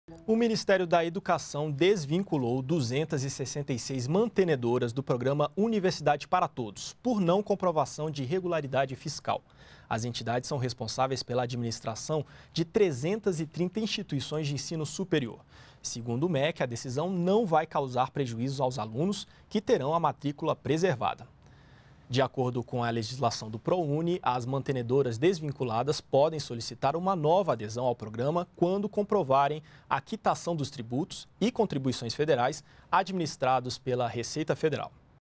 Mais de 260 instituições têm vínculo rompido com o Prouni por irregularidade fiscal
NBR NOTÍCIAS - 20.05.13: Nesta segunda-feira (20), 266 instituições deixaram de fazer parte do Programa Universidade para Todos (Prouni). De acordo com o Ministério da Educação (MEC), as entidades, responsáveis pela administração de 330 instituições de ensino superior, não comprovaram regularidade fiscal. Segundo as normas do Prouni, as mantenedoras desvinculadas podem solicitar uma nova adesão ao programa quando indicarem a quitação dos tributos e contribuições federais administrados pela Receita Federal. A decisão não vai causar prejuízos aos estudante, que terão a matrícula preservada.